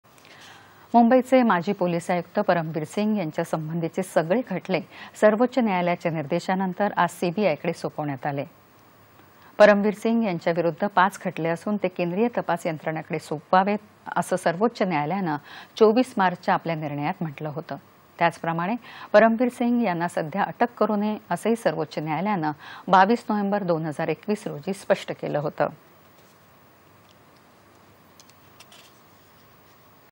परमवीर सिंग यांच्या संबंधिचे सगळे खटले, सीबीआयकडे सोपवण्यात आले.
दि.१३.०४.२०२२ :
 मुंबईचे माजी पोलिस आयुक्त परमवीर सिंग यांच्या संबंधिचे सगळे खटले, सर्वोच्च  न्यायालयाच्या निर्देशानंतर, आज सीबीआयकडे सोपवण्यात आले.  परमवीर सिंग यांच्याविरुध्द पाच खटले असून, ते केंद्रीय तपास यंत्रणेकडे सोपवावेत, असं सर्वोच्च न्यायालयानं, २४ मार्चच्या आपल्या निर्णयात म्हटलं होतं, त्याचप्रमाणे परमवीर सिंह यांना सध्या अटक करु नये, असंही सर्वोच्च न्यायालयानं ,२२ नोव्हेंबर २०२१ रोजी स्पष्ट केलं होतं.